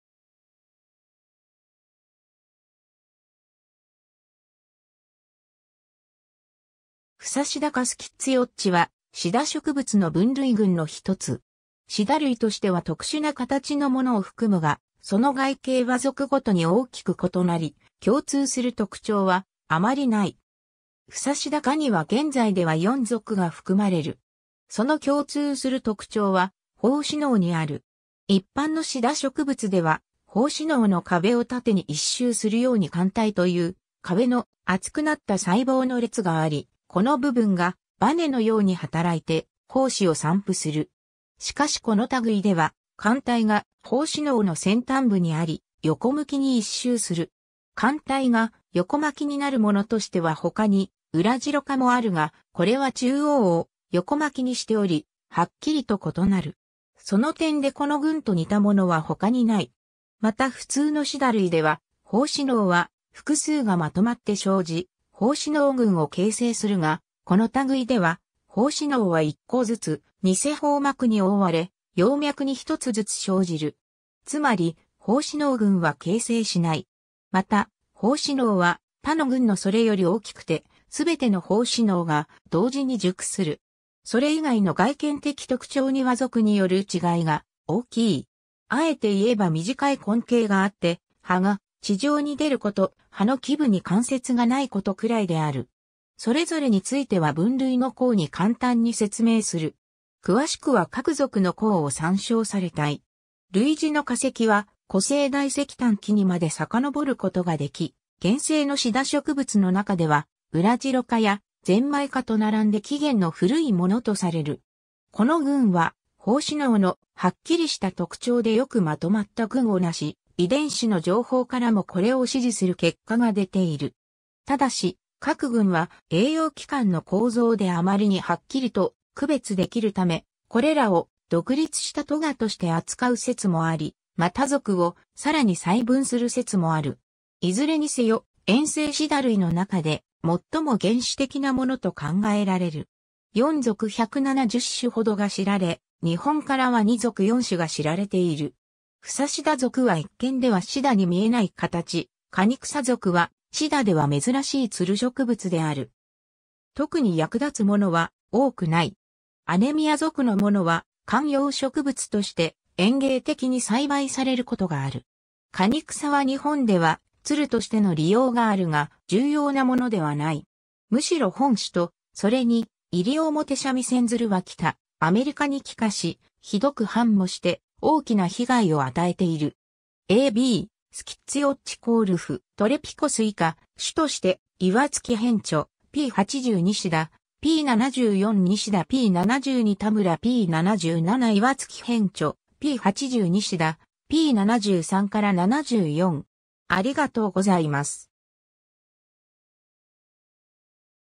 フサシダ科
フサシダ科 Schizaeaceae は、シダ植物の分類群の一つ。
シダ類としては特殊な形のものを含むが、その外形は属ごとに大きく異なり、共通する特徴はあまりない。
 フサシダ科には現在では4属が含まれる。
その共通する特徴は胞子嚢にある。
一般のシダ植物では、胞子嚢の壁を縦に一周するように環帯という壁の厚くなった細胞の列があり、この部分がバネのように働いて胞子を散布する。
しかしこの類では環帯が胞子嚢の先端部にあり、横向きに一周する。
環帯が横巻きになるものとしては他にウラジロ科もあるが、これは中央を横巻きにしており、はっきりと異なる。
その点でこの群と似たものは他にない。
 また普通のシダ類では胞子嚢は複数がまとまって生じ、胞子嚢群を形成するが、この類では胞子嚢は一個ずつ偽胞膜に覆われ、葉脈に1つずつ生じる。
つまり胞子嚢群は形成しない。
また胞子嚢は他の群のそれより大きくて、全ての胞子嚢が同時に熟する（これを斉熟と言い、これも原始的特徴とされる）。
 それ以外の外見的特徴には属による違いが大きい。
あえて言えば短い根茎があって葉が地上に出ること、葉の基部に関節がないことくらいである。
それぞれについては分類の項に簡単に説明する。
詳しくは各属の項を参照されたい。
 類似の化石は古生代石炭紀にまで遡ることができ、現生のシダ植物の中ではウラジロ科やゼンマイ科と並んで起源の古いものとされる。
 この群は胞子嚢のはっきりした特徴でよく纏まった群を成し、遺伝子の情報からもこれを支持する結果が出ている。
ただし、各群は栄養器官の構造であまりにはっきりと区別できるため、これらを独立した科として扱う説もあり、また属をさらに細分する説もある。
いずれにせよ、縁性シダ類の中でもっとも原始的なもの（薄嚢シダ中で、とも）と考えられる。
 4属170種ほどが知られ、日本からは2属4種が知られている。
フサシダ属は一見ではシダに見えない形、カニクサ属はシダでは珍しい蔓植物である。
 特に役立つものは多くない。
アネミア属のものは観葉植物として園芸的に栽培されることがある。
カニクサは日本では蔓としての利用があるが、重要なものではない。
むしろ本種と、それにイリオモテシャミセンヅルは北アメリカに帰化し、ひどく繁茂して大きな被害を与えている。
 ^ a b Schizaeaceae Kaulf. Trpicos ^ 以下、主として岩槻編著(1992),p.80 ^ 西田(1997),p.74 ^ 西田(1997)p.72 ^ 田村(1999)p.77 ^ 岩槻編著(1992),p.80 ^ 西田(1997),p.73-74